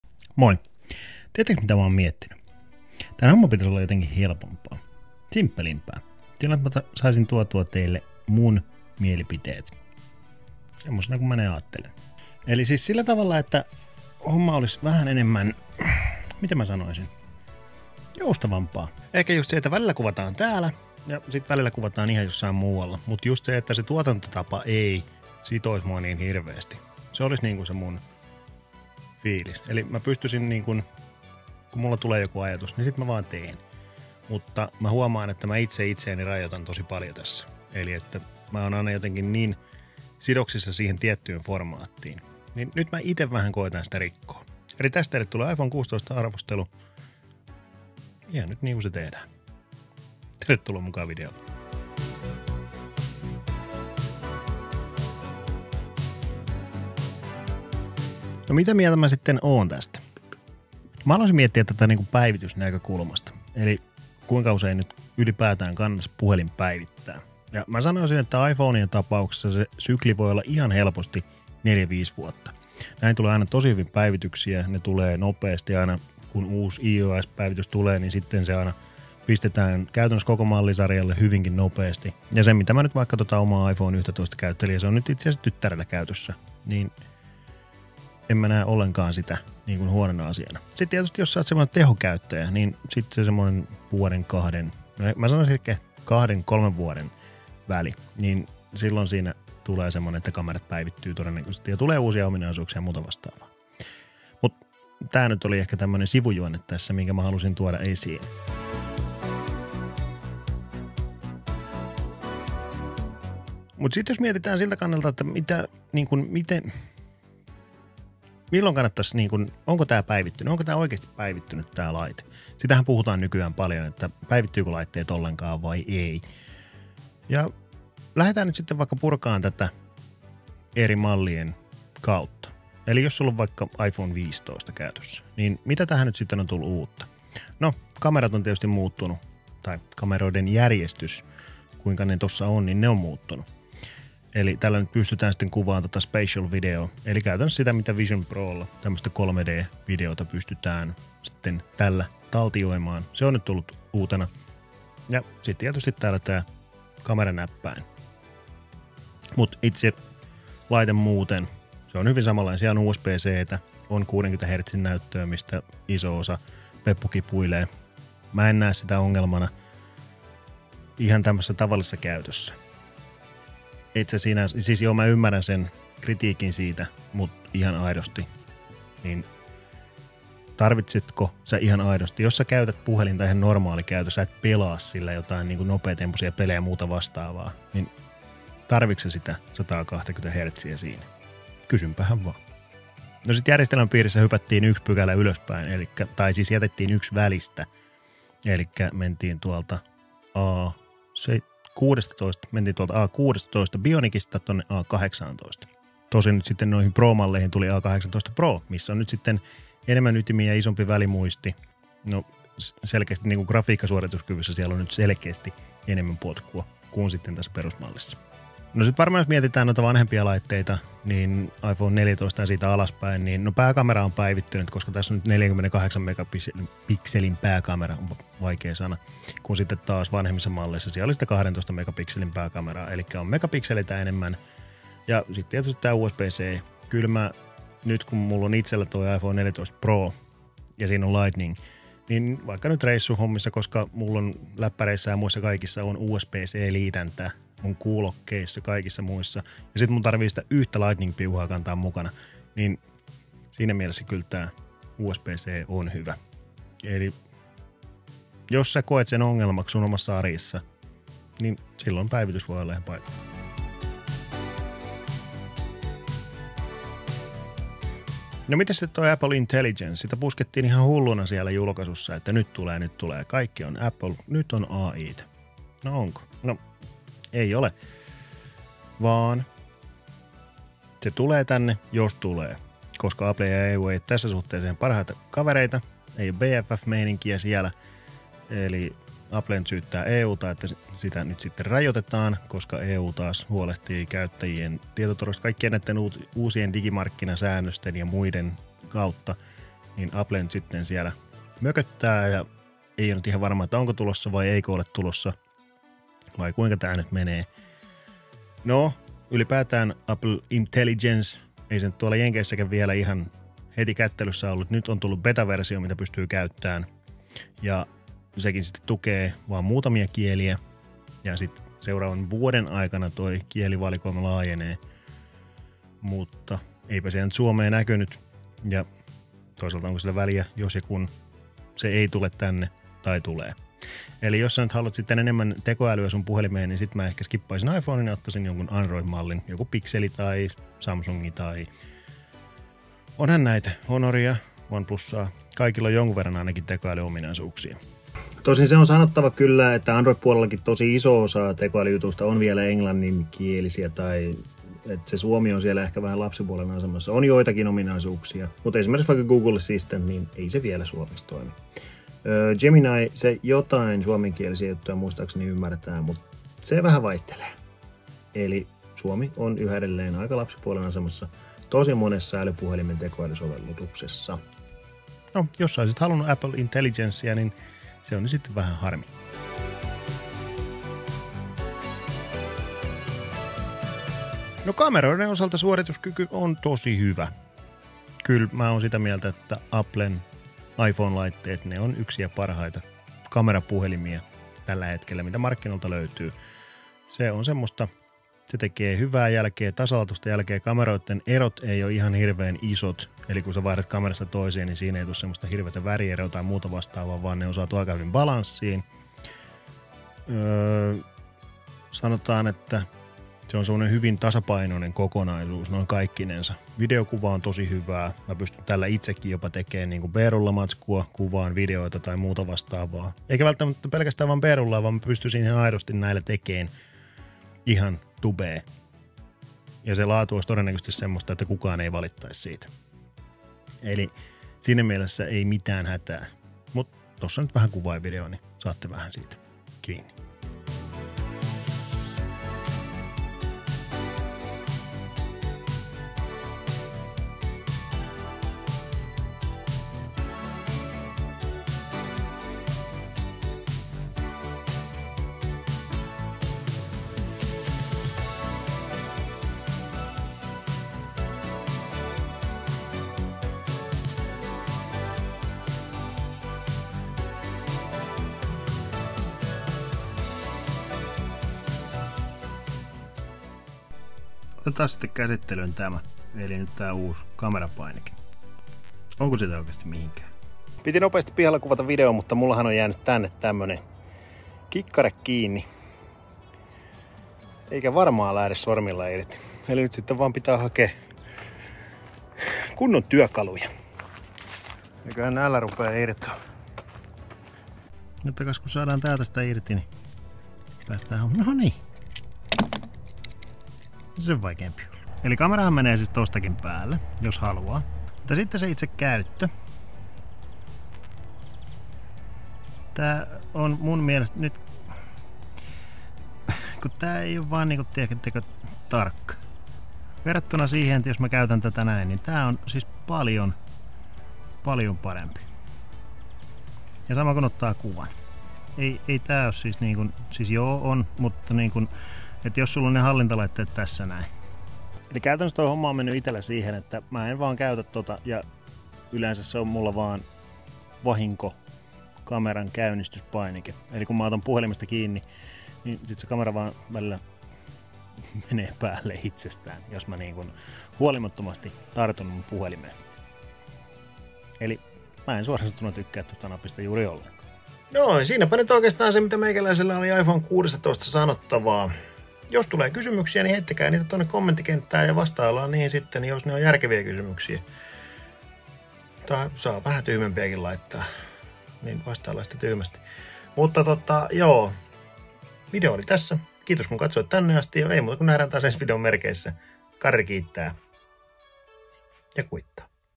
Onko päivitys vai ei? - iPhone 16 arvostelu
Tervetuloa Tekniikkaluolaan! Katsotaan, onko iPhone 16 todellakin lähes Pro-versio. Kuinka usein puhelinta kannattaa päivittää? iPhone 16 tarjoaa muutamia uudistuksia verrattuna edellisiin malleihin, kuten uuden järjestelmäpiirin ja Spatial-videon kuvausmahdollisuuden. Lisäksi USB-C-liitäntä ja Apple Intelligence tekoälyavustaja ovat mukana. Kameroiden laatu on perinteisesti Applen vahvuus, mutta uusi kamerapainike herättää ristiriitaisia tunteita. Katsotaan, onko iPhone 16 päivityksen arvoinen vai ei. Katso video ja kerro mielipiteesi!

Testilaitteen on toimittanut DNA ja se palautuu testien jälkeen heille.